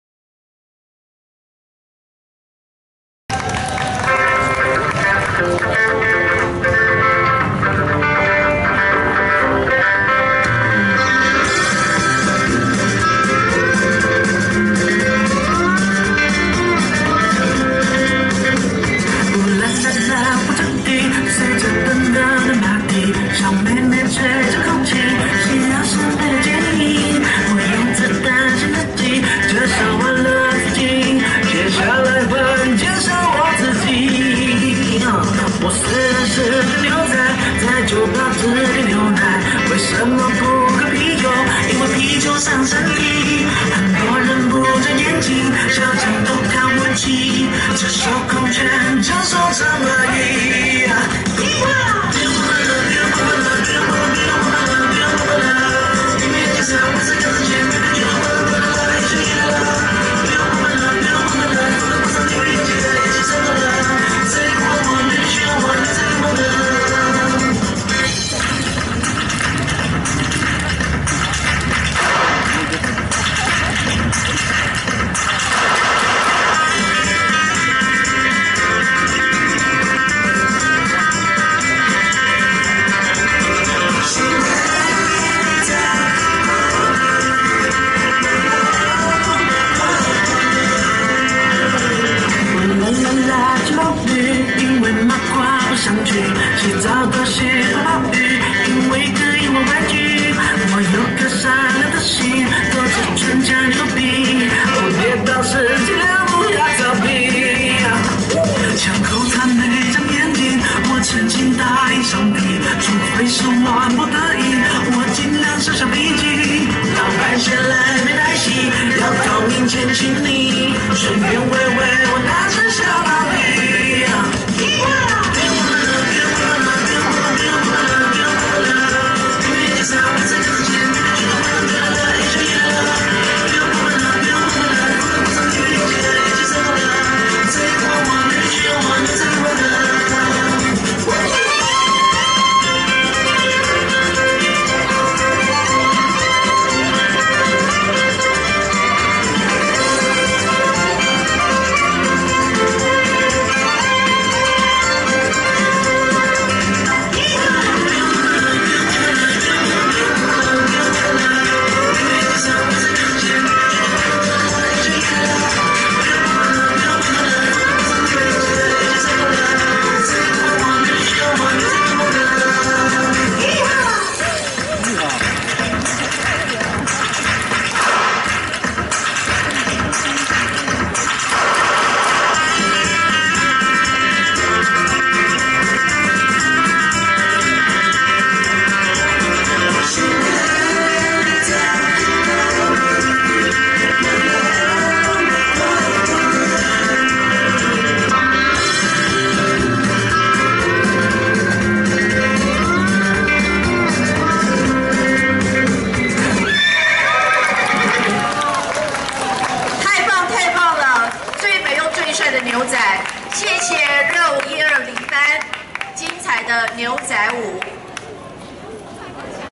【金山灣區雙十國慶升旗典禮】熱舞120班 牛仔舞
金山灣區中華民國國慶委員會10月5日在聖他克拉拉縣政府廣場隆重舉行113年雙十國慶升旗典禮，由主任委員葉雲河主持，駐舊金山辦事處處長賴銘琪、金山灣區文教中心主任莊雅淑及當地多名主流政要、民選官員和僑學各界人士近千人皆受邀出席，共同慶祝中華民國生日，場面莊嚴熱烈，充分展現海外僑胞對中華民國的深厚情感與支持。